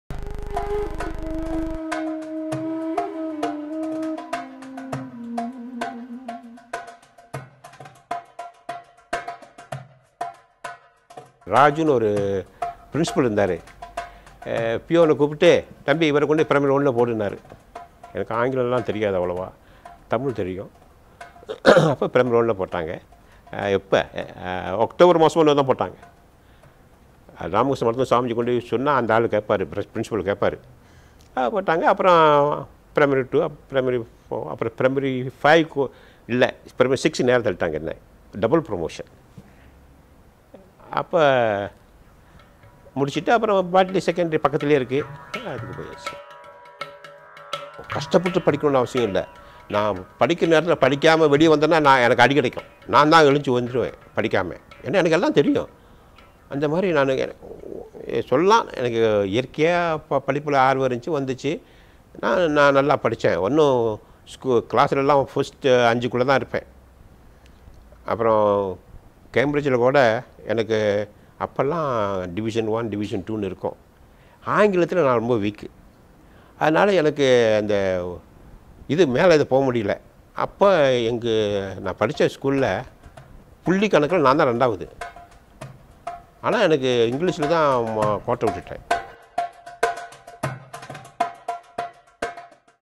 PON2of6_A NATURAL
There were no tutors in Ramakrishna Mission to guide Ponnusamy academically when he got back from school. However, he proudly remembers being one of the top students in class during his school days.  He believes what allowed him to cope well was his interest in acquiring more knowledge. Coming from India where English was not the first language, the young Ponnusamy struggled to gain good command over the language while excelling in other subjects.